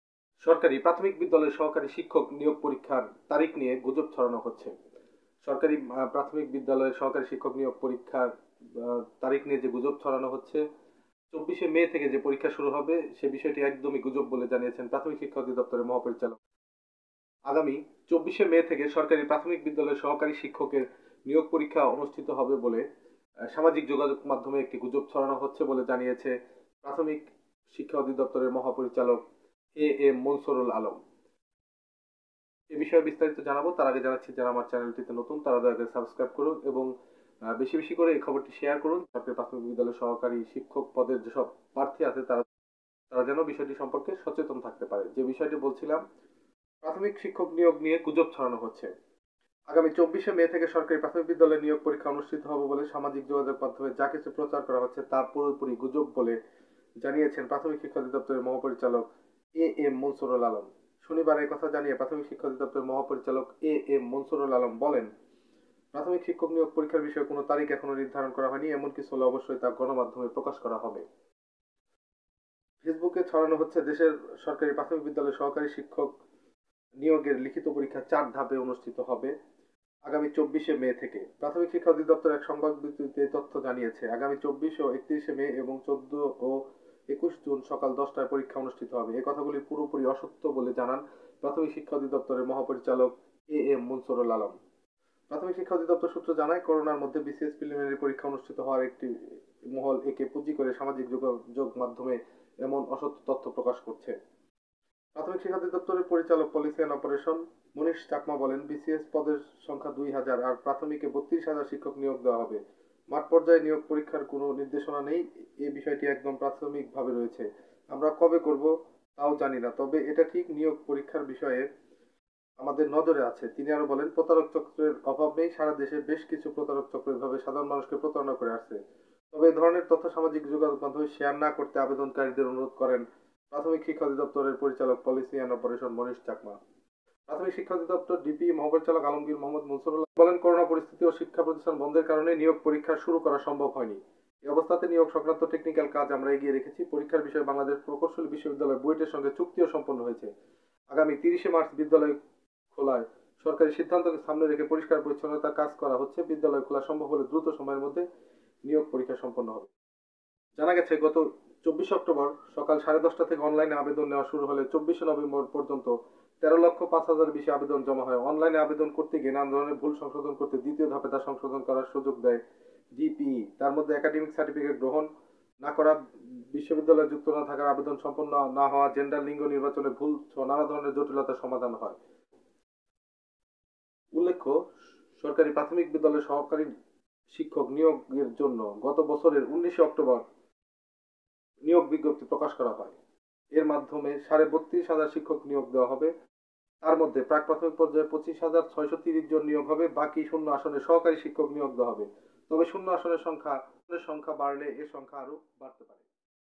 প্রাথমিক শিক্ষক নিয়োগ পরীক্ষা নিয়ে গুজব ছড়ানো হচ্ছে
আগামী ২৪ মে থেকে সরকারি প্রাথমিক বিদ্যালয়ে নিয়োগ পরীক্ষা অনুষ্ঠিত হবে বলে সামাজিক যোগাযোগ মাধ্যমে যা কিছু প্রচার করা হচ্ছে, তা পুরোপুরি গুজব বলে জানিয়েছেন প্রাথমিক শিক্ষা অধিপ্তরের মহাপরিচালক এ এম মনসুরুল আলম।
শনিবার এ কথা জানি‌য়ে প্রাথমিক শিক্ষা অধিপ্তরের মহাপরিচালক এ এম মনসুরুল আলম বলেন, প্রাথমিক শিক্ষক নিয়োগ পরীক্ষার বিষয়ে কোনো তারিখ এখনো নির্ধারণ করা হয়নি। এমন কিছু হলে অবশ্যই তা গণমাধ্যমের মাধ্যমে প্রকাশ করা হবে।
ফেসবুকে ছড়ানো হচ্ছে, দেশের সরকারি প্রাথমিক বিদ্যালয়ের সহকারী শিক্ষক নিয়োগের লিখিত পরীক্ষা ৪ ধাপে শুরু হবে আগামী ২৪ মে। প্রাথমিক শিক্ষা অধিদপ্তর এক সংবাদ বিজ্ঞপ্তিতে এই তথ্য জানিয়েছে। আগামী ২৪ ও ৩১ মে এবং ১৪ ও ২১ জুন সকাল ১০টায় পরীক্ষা অনুষ্ঠিত হবে। এই তথ্যগুলো পুরোপুরি অসত্য বলে জানান প্রাথমিক শিক্ষা অধিপ্তরের মহাপরিচালক এ এম মনসুরুল আলম।
প্রাথমিক শিক্ষা অধিদপ্তর সূত্র জানায়, করোনার মধ্যে বিসিএসের প্রিলিমিনারি পরীক্ষা অনুষ্ঠিত হওয়ায় একটি মহল একে পুঁজি করে সামাজিক যোগাযোগ মাধ্যমে এমন অসত্য তথ্য ছড়াচ্ছে।
প্রাথমিক শিক্ষা অধিদপ্তরের পরিচালক (পলিসি অ্যান্ড অপারেশন) মনীষ চাকমা বলেন, বিসিএসে পদের সংখ্যা ২ হাজার আর প্রাথমিকে ৩২ হাজার শিক্ষক নিয়োগ দেয়া হবে। মাঠ পর্যায়ে নিয়োগ পরীক্ষার কোনো নির্দেশনা নেই। এ বিষয়টি একদম প্রাথমিক স্তরে রয়েছে। আমরা কবে করবো তাও জানি না। তবে এটাও ঠিক নিয়োগ পরীক্ষার বিষয়ে গুজবের বিষয়টি আমাদের নজরে আছে।
তিনি আরো বলেন, প্রতারক চক্রের অভাব নেই। সারা দেশে বেশকিছু প্রতারক চক্র এভাবে সাধারণ মানুষকে প্রতারণা করে আসছে। তবে এ ধরনের তথ্য সামাজিক যোগাযোগ মাধ্যমে শেয়ার না করতে আবেদনকারীদের অনুরোধ করেন প্রাথমিক শিক্ষা অধিদপ্তরের পরিচালক (পলিসি অ্যান্ড অপারেশন) মনীষ চাকমা।
ডিপিই সূত্রে জানা গেছে, গত ২৫ অক্টোবর সকাল সাড়ে ১০টা থেকে অনলাইনে আবেদন নেয়া শুরু হলে ২৪ নভেম্বর পর্যন্ত ১৩ লাখ ৫ হাজারের বেশি আবেদন জমা হয়। অনলাইন আবেদন করতে গিয়ে নানা ধরনের ভুল সংশোধন করতে ২ ধাপে তা সংশোধন করতে সুযোগ দেয় ডিপিই। তার মধ্যে একাডেমিক সার্টিফিকেট গ্রহণ না করা, বিশ্ববিদ্যালয় যুক্ত না থাকায় আবেদন সম্পন্ন না হওয়া, জেন্ডার (লিঙ্গ) নির্বাচনে ভুলসহ নানা ধরনের জটিলতা সমাধান করা হয়।
প্রাথমিক শিক্ষা অধিদপ্তরের (ডিপিই) মহাপরিচালক আলমগীর মুহাম্মদ মনসুরুল আলম বলেন, শিক্ষা প্রতিষ্ঠান খোলার পর প্রাথমিক বিদ্যালয়ে শিক্ষক নিয়োগ পরীক্ষা শুরু করার চিন্তা ভাবনা করা হচ্ছে। এজন্য সব ধরনের প্রস্তুতি প্রায় শেষ পর্যায়ে রয়েছে।
মহাপরিচালক বলেন, করোনা পরিস্থিতি ও শিক্ষা প্রতিষ্ঠান বন্ধের কারণে নিয়োগ পরীক্ষা শুরু করা সম্ভব হয়নি। এ অবস্থাতেও নিয়োগ সংক্রান্ত টেকনিক্যাল কাজ আমরা এগিয়ে রেখেছি। পরীক্ষার বিষয়ে বাংলাদেশ প্রকৌশল বিশ্ববিদ্যালয়ের (বুয়েট) সঙ্গে চুক্তিও সম্পন্ন হয়েছে। আগামী ৩০ মার্চ বিদ্যালয় খোলার সরকারি সিদ্ধান্তকে সামনে রেখে পরিষ্কার পরিচ্ছন্নতার কাজ করা হচ্ছে। বিদ্যালয় খোলা সম্ভব হলে দ্রুত সময়ের মধ্যে নিয়োগ পরীক্ষা শুরু করা হবে।
তিনি আরো বলেন, সারাদেশে আমাদের ১৩ লাখের বেশি নিয়োগের জন্য আবেদন রয়েছে। করোনা পরিস্থিতির মধ্যে এত বিশাল সংখ্যক প্রার্থীদের নিয়োগ পরীক্ষা নেয়াটা কঠিন হয়ে দাঁড়িয়েছে। এ কারণে প্রস্তুতি থাকলেও ঝুঁকি এড়াতে এখন পর্যন্ত পরীক্ষার সময় চূড়ান্ত করা হয়নি। করোনা পরিস্থিতি নিয়ন্ত্রণে এলে এ পরীক্ষা শুরু করা হবে।